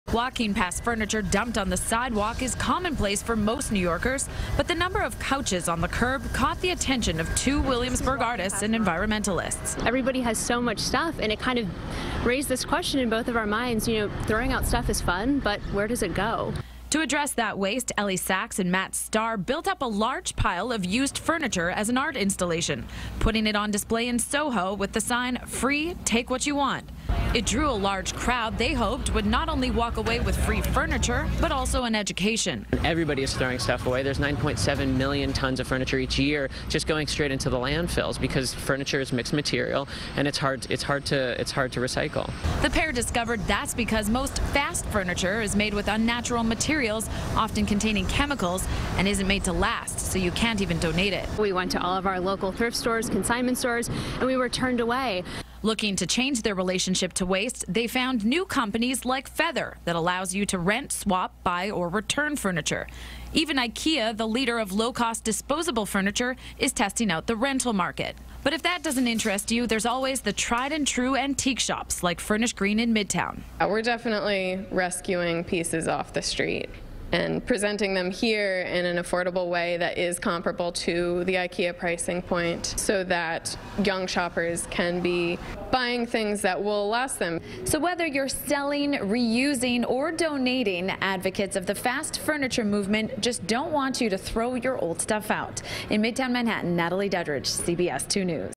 Brooklyn Artists Furniture Medium Their Message
As part of an art installation, Ellie Sachs and Matt Starr built up a large pile of used furniture, putting it on display in SoHo with the sign saying “Free, take what you want.” CBS2's Natalie Duddridge reports.